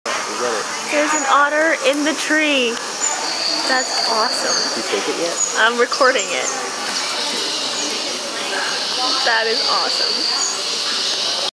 Bonx Zoo Otter in a Tree!
I never new that otters could climb trees... but this makes me want one all the more!